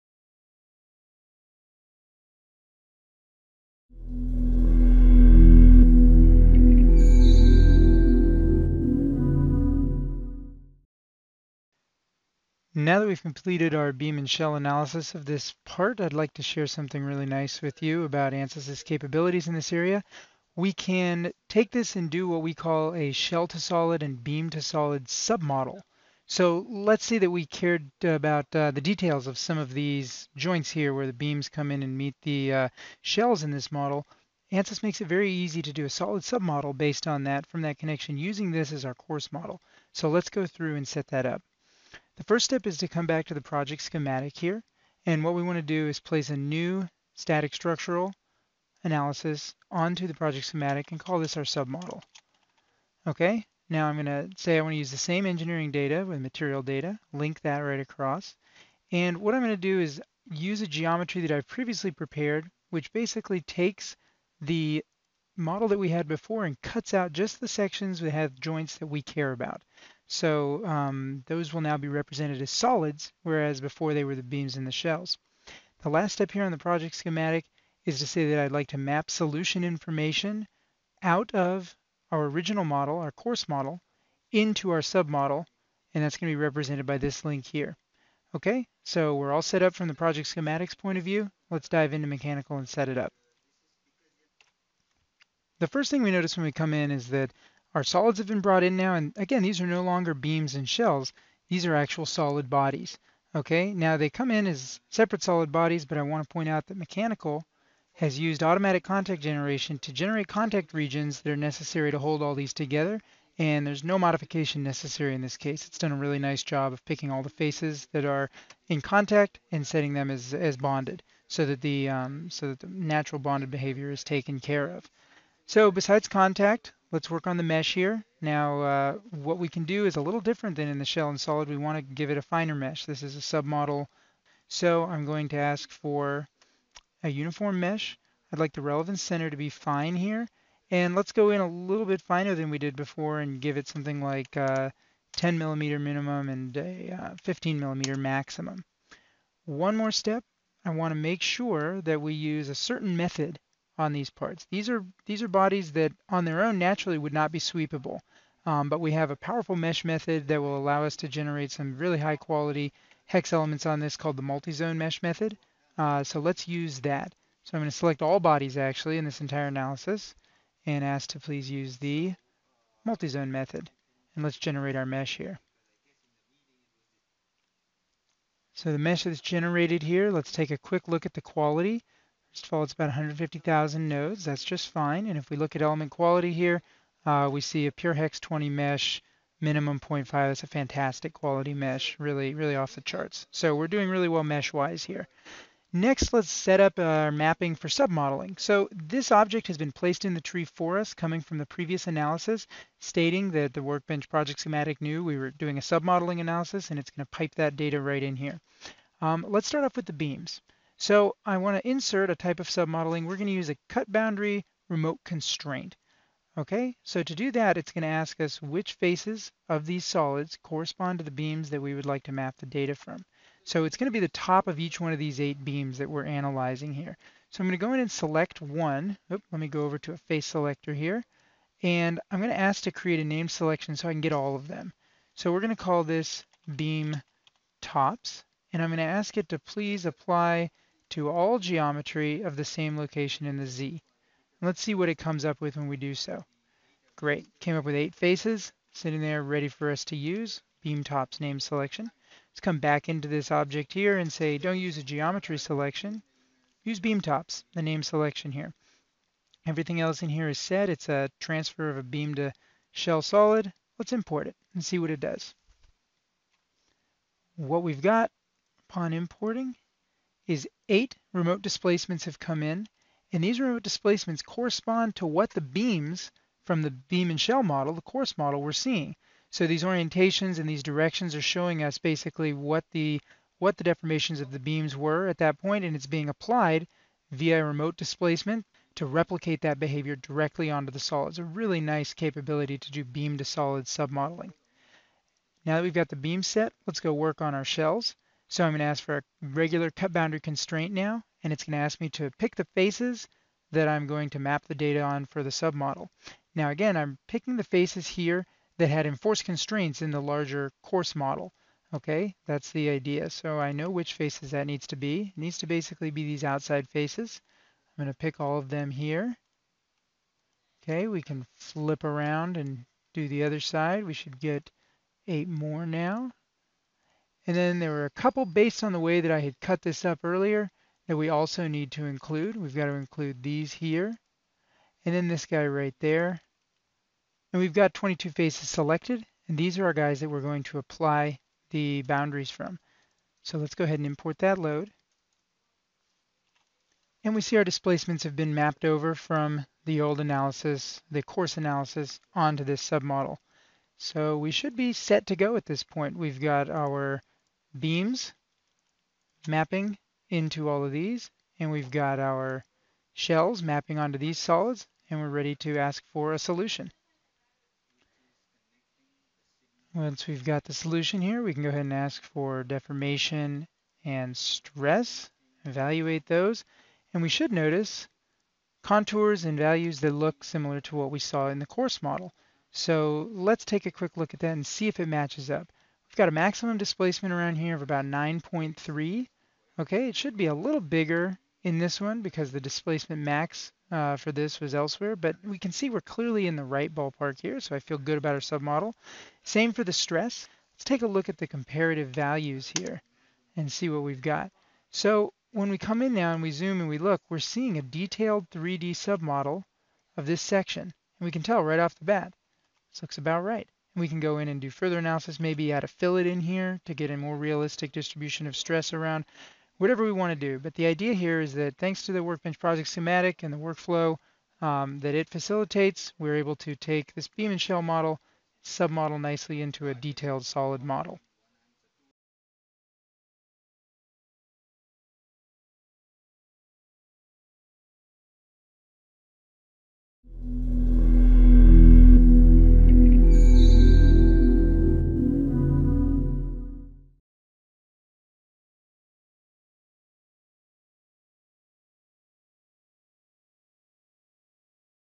Beam and Shell Submodeling with ANSYS Mechanical [Tutorial]
Starting from a global beam and shell model, this tutorial video shows how to easily and quickly take results into a detailed solid model to evaluate details around connections and look at the effects of stresses on joints.